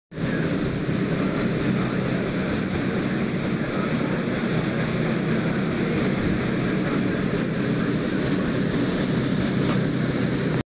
Alaska - Paddlewheel
Paddlewheel on the Discover III, which we rode up and down the Chena River near Fairbanks.